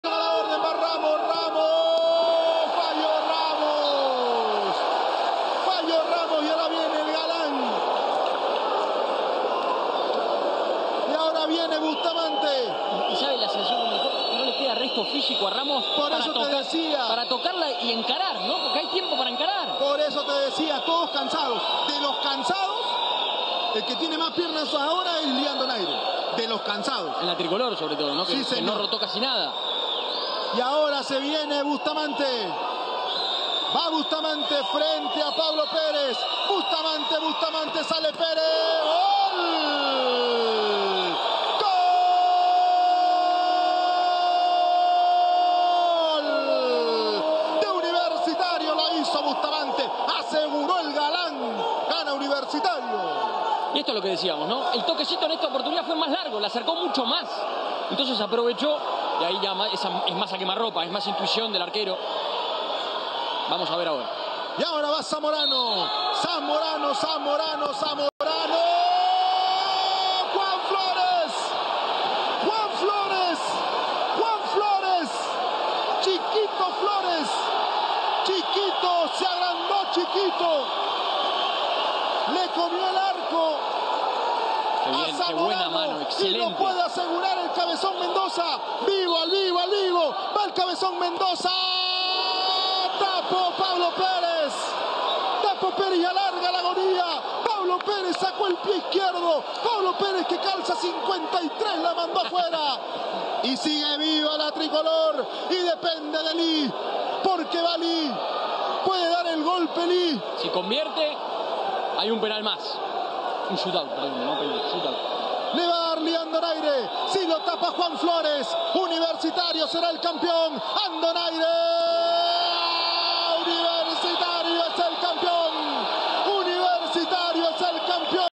Copa Leyendas: así fue la definición de shoot outs entre Universitario y La Tricolor
Gracias al talento de Juan Flores 🧤, y a la efectividad de Diego Bustamante 🔥, Universitario 🟠 venció por shoot outs a La Tricolor 🔵⚪🔴 y se coronó campeón de la Copa Leyendas 🏆.